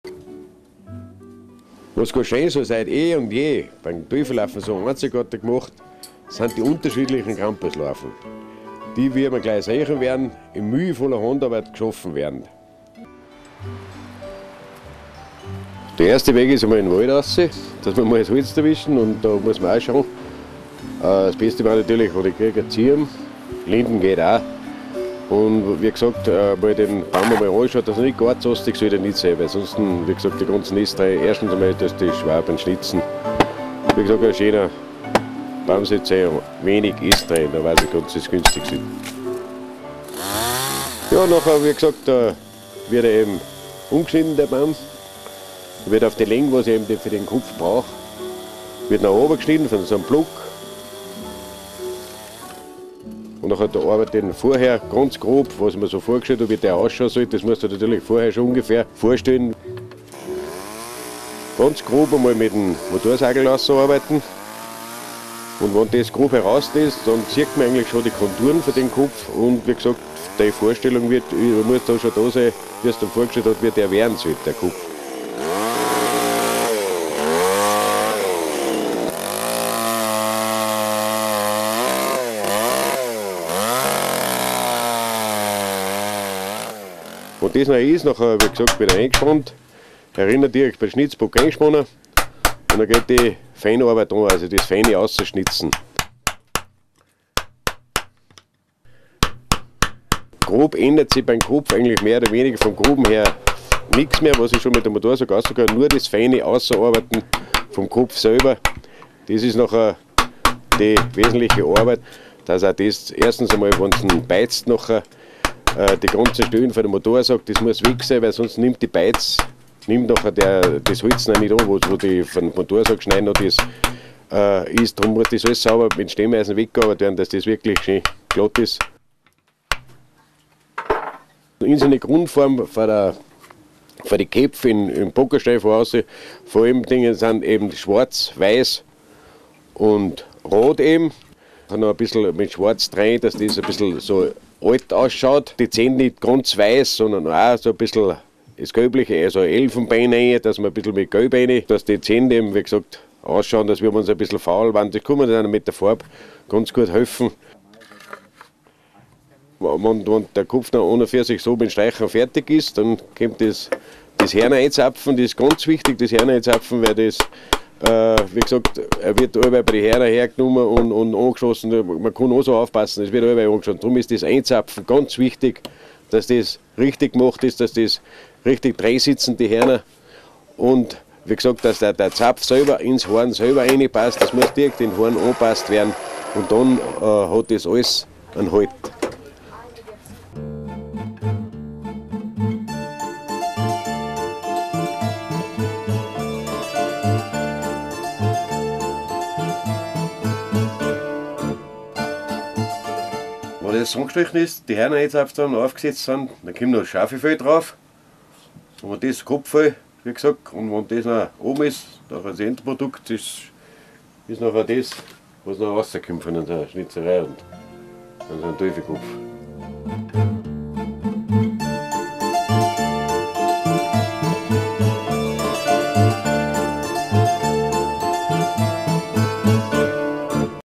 Krampuslauf in Gastein - Der Krampuskopf
Für einen Krampuskopf werden viele Arbeitsschritte benötigt: Holz einbringen, den Holzklotz zuschneiden, den Krampuskopf schnitzen, die Hörner einzapfen und das Kopffell anpassen, dann ist der Kopf für den Gasteiner Krampuslauf fertig. Brauchtum in Salzburg in seiner ursprünglichsten Form.